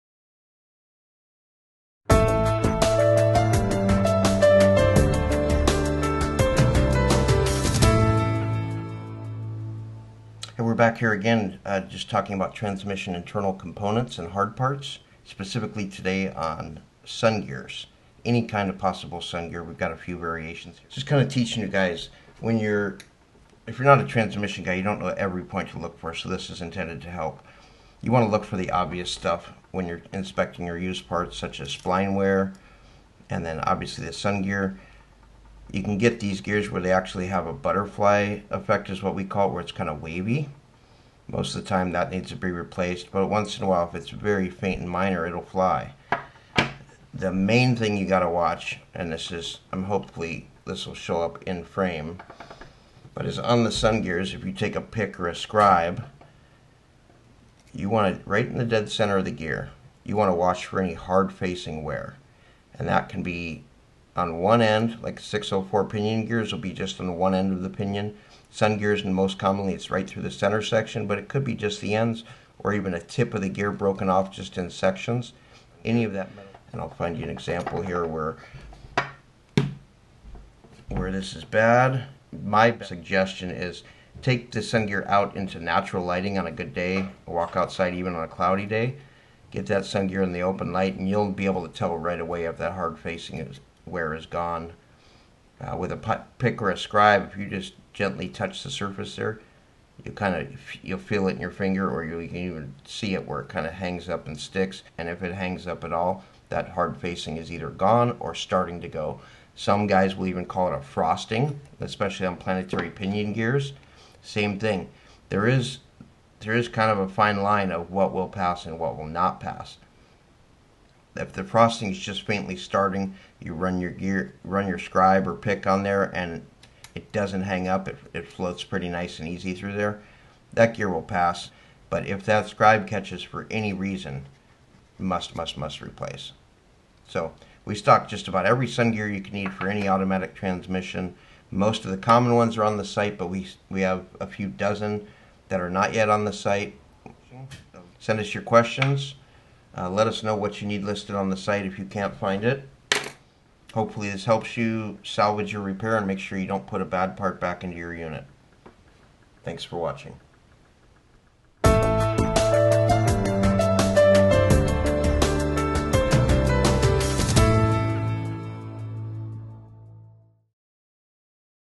Transmission Sun Gear Inspection (Is My Sun Gear Bad?) Pitting Explained!
CASCADE.PARTS

Here is a link to our Sun Gear page on the website!